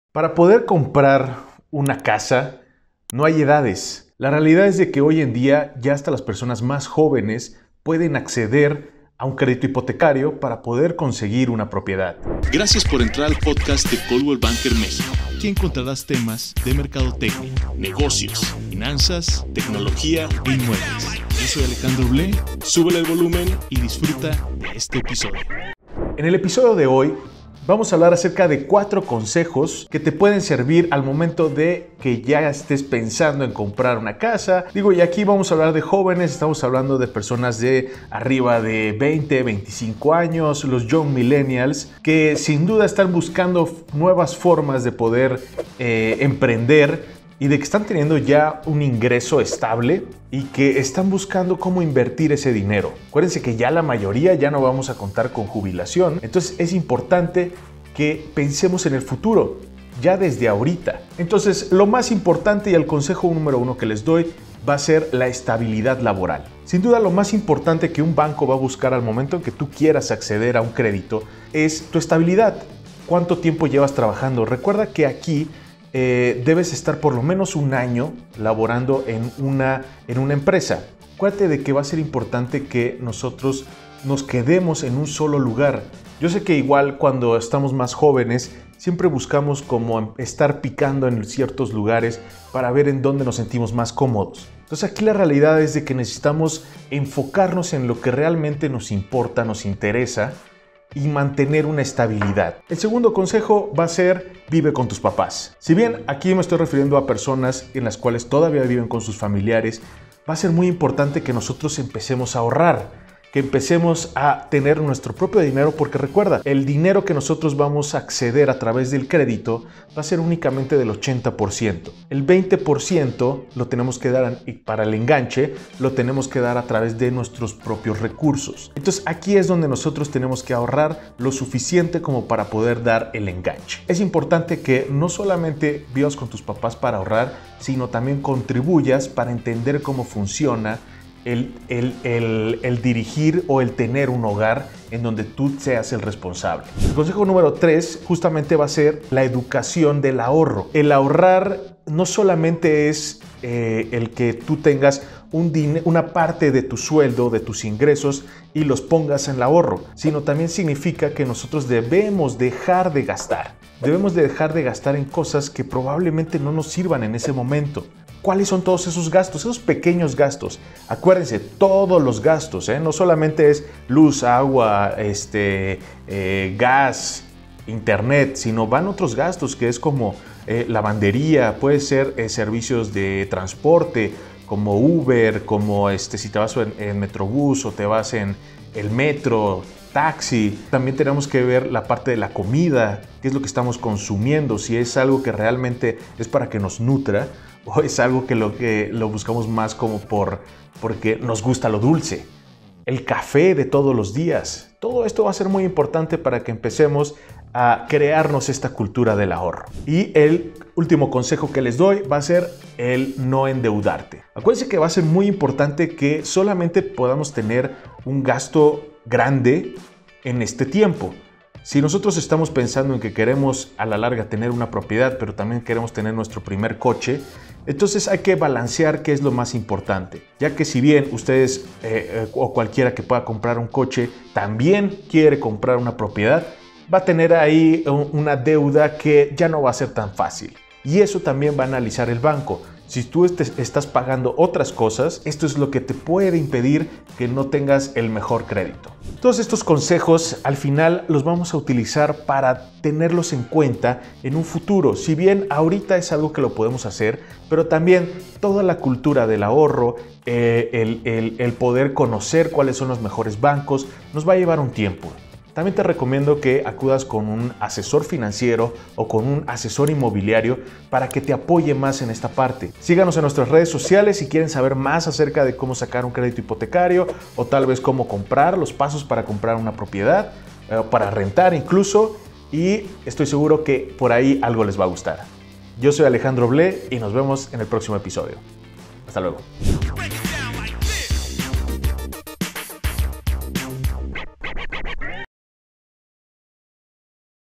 CRÉDITOS HIPOTECARIOS PARA LA CHAVIZA 😎 ALEJANDRO BLÉ
🤔Si eres de las personas que piensa que por ser joven no lograrás tener tu casa propia, este podcast es para ti. Conoce 4 factores esenciales para obtener el mejor crédito hipotecario y lograr esa gran independencia. 🫶